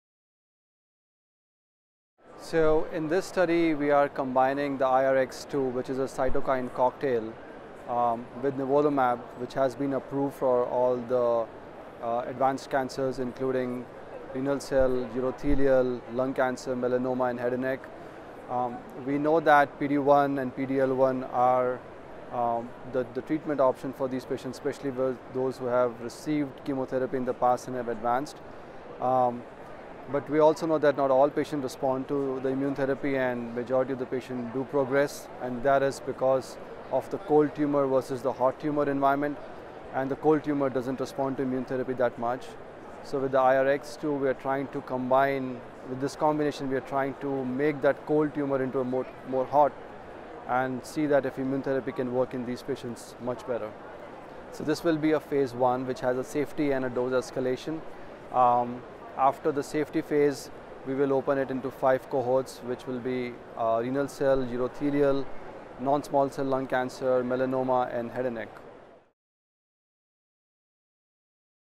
ASCO 2019: Rohit K. Jain, MD, MPH Abstract - TPS2662
The IRX-2 regimen combined with nivolumab in recurrent/metastatic solid tumors: A phase 1b study to evaluate the safety, determine recommended phase 2 dose (RP2D), and investigate the biologic and clinical activity.